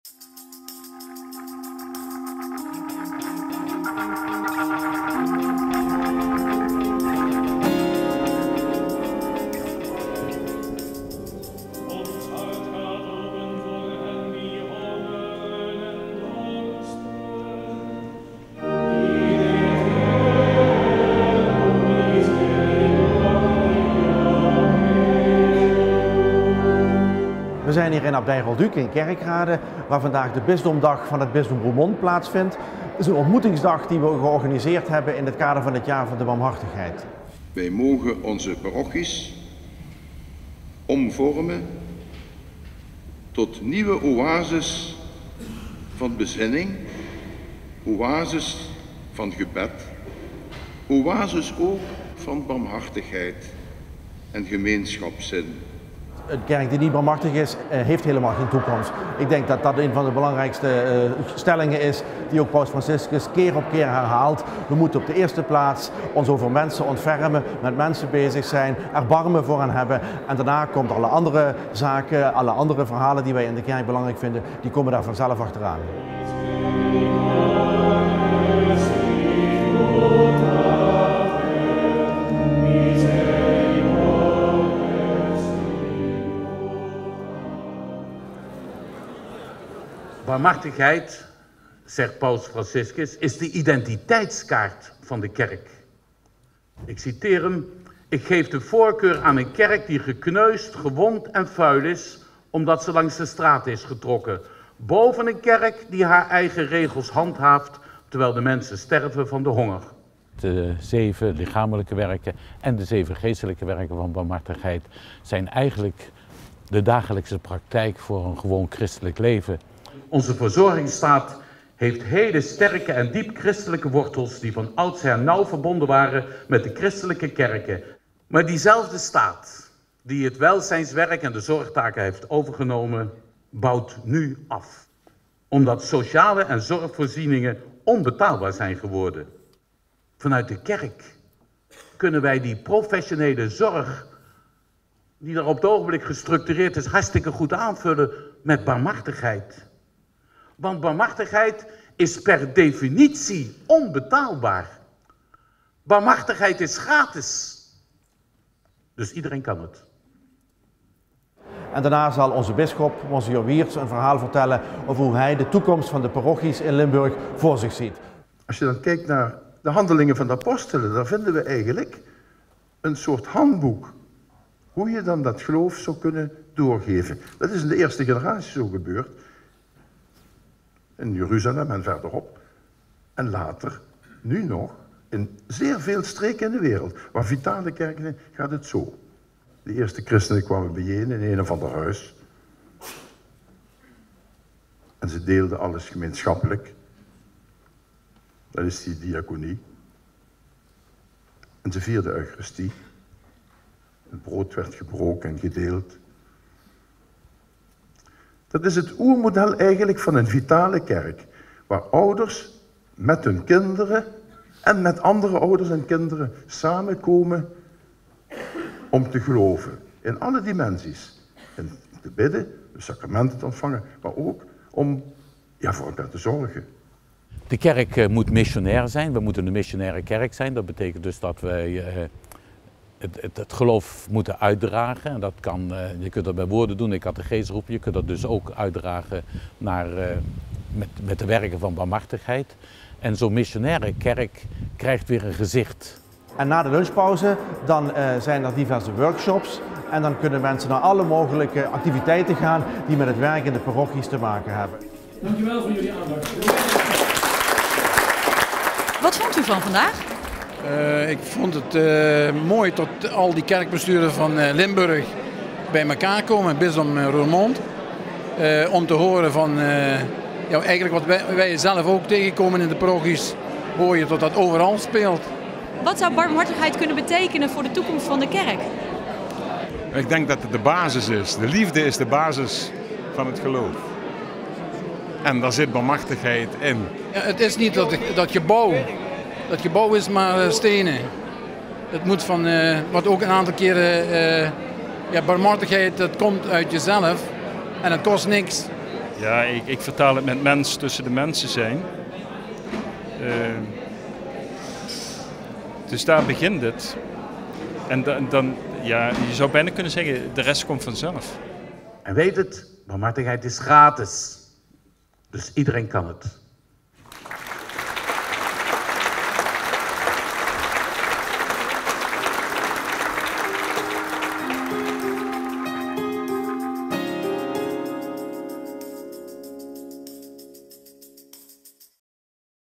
Heilig Jaar - Bisdomdag Bisdom Roermond
“Wij mogen onze parochies omvormen tot nieuwe oases van bezinning. Oases van gebed. Oases ook van barmhartigheid en gemeenschapszin.” Dit zei Mgr. Wiertz van Roermond tijdens de bisdomdag op zaterdag 15 oktober in Abdij Rolduc. Het bisdom hield deze dag in het jaar van de barmhartigheid. Katholiekleven.nl was erbij en maakte deze film.

Mgr. Wiertz vertelt in de film: “De eerste christenen kwamen bijeen in één of ander huis. En ze deelden alles gemeenschappelijk, dat is de diaconie. En ze vierden eucharistie, het brood werd gebroken en gedeeld. Dat is het oermodel eigenlijk van een vitale kerk, waar ouders met hun kinderen én met andere ouders en kinderen samenkomen om te geloven. In alle dimensies: in het bidden, de sacramenten te ontvangen, maar ook om voor elkaar te zorgen.”

Naast Mgr. Wiertz sprak pater Wim van Meijgaarden s.s.s. op de bisdomdag zaterdag 15 oktober in Abdij Rolduc. Hij is Hoofdaalmoezenier van Sociale Werken in het bisdom Roermond. Hij zegt: “De zeven lichamelijke werken en de zeven geestelijke werken van barmhartigheid zijn eigenlijk de dagelijkse praktijk voor een gewoon christelijk leven.”